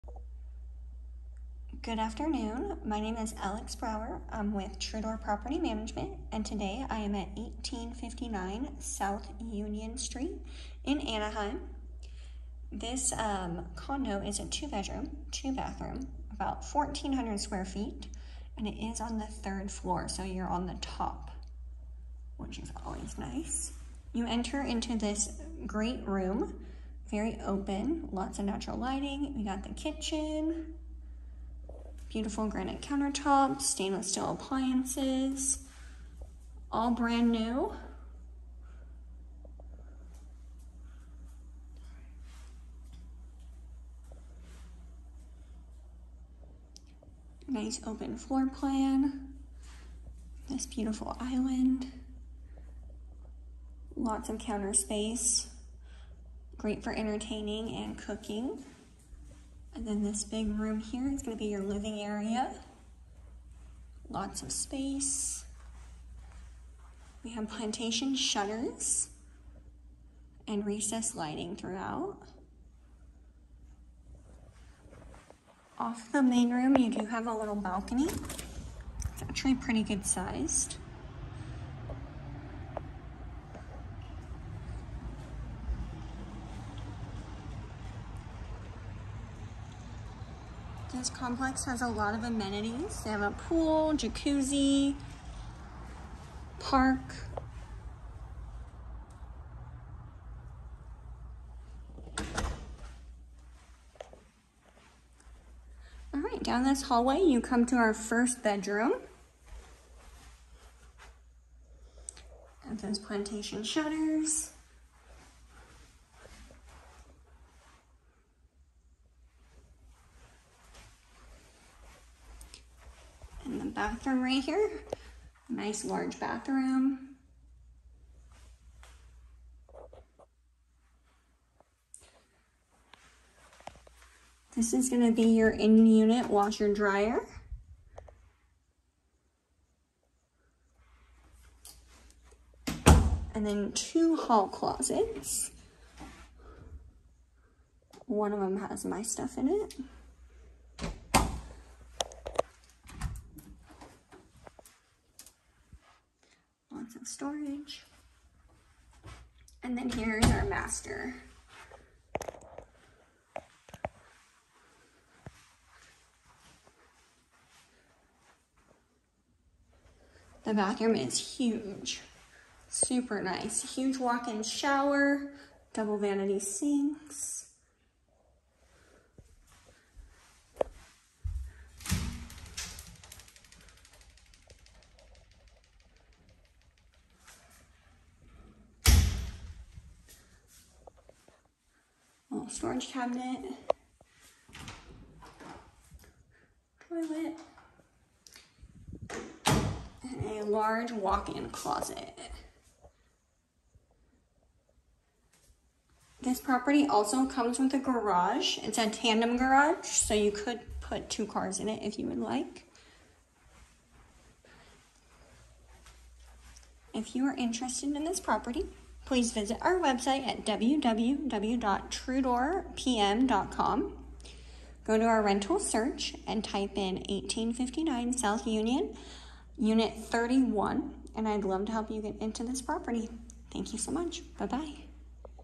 FOR RENT: 1859 S. Union St #31
Brand New Two Bedroom Condo in Anaheim!
1859 S. Union St #31
2 bed / 2 bath / 1,412 sqft

Upscale living in the gorgeous Alia in the heart of Anaheim’s Platinum Triangle. This luxury flat features 2 bedrooms and 2 full baths with a 2-car tandem detached garage. Single level with an outdoor deck. Step inside this sought-after, open concept floor plan and enjoy this wonderful chef's kitchen with white shaker cabinets, stainless steel appliances, granite counters, living room and a dining room. Beyond the living room is a slider that leads to a private balcony. The primary bedroom includes an en-suite bathroom with a dual sink vanity, a step-in shower with glass enclosure and a walk-in closet and an in-unit laundry closet. The association amenities include a resort like pool, spa, outdoor BBQ area and park like areas, including a tot lot. Conveniently located close to the Angel's Stadium, the Honda Center, Disneyland and UCI Medical Center. Quick and easy access to the 5 and 57 freeways, facilitating seamless connectivity to nearby cities and destinations.

Home is available for an immediate move in with a 12 month lease and security deposit on approved credit. Property is leased in “as shown condition” unless otherwise advertised. Renter’s insurance or Tenant Liability Insurance is required.

This property is eligible for Rhino deposit coverage in lieu of security deposit. Free quotes available. For more information ask me!

Please submit pets for approval

$49 application fee per person. Incomplete applications will not be considered.
1.5% monthly technology fee
$500 deposit for each approved pet, plus additional fees.